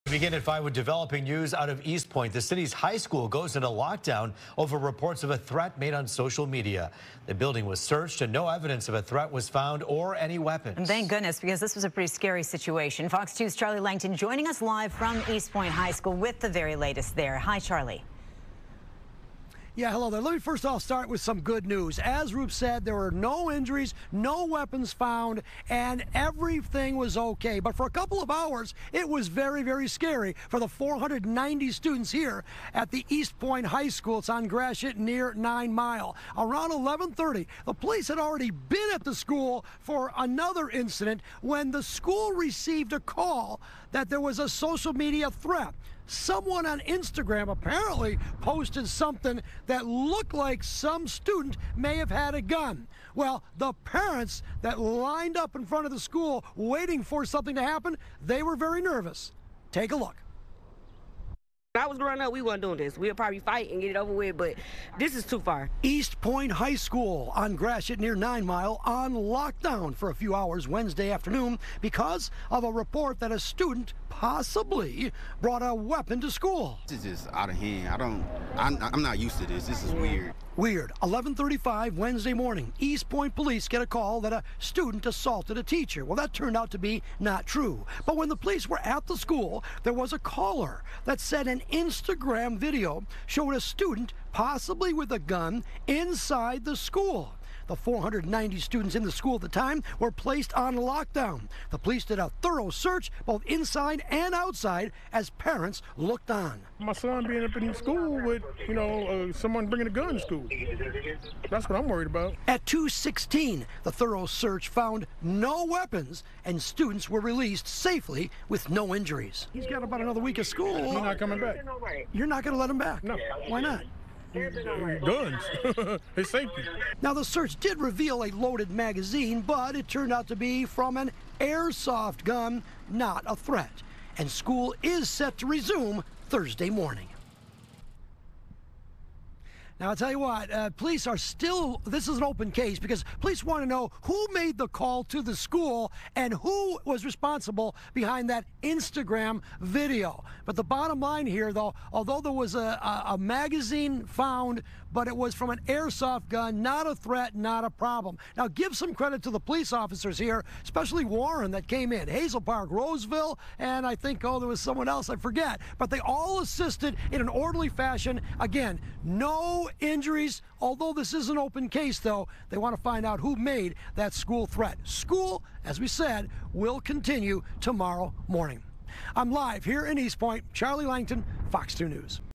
No guns or evidence found after threat of gun at Eastpointe High School
Police said they searched Eastpointe High School after a reported threat of a person with a gun inside the school.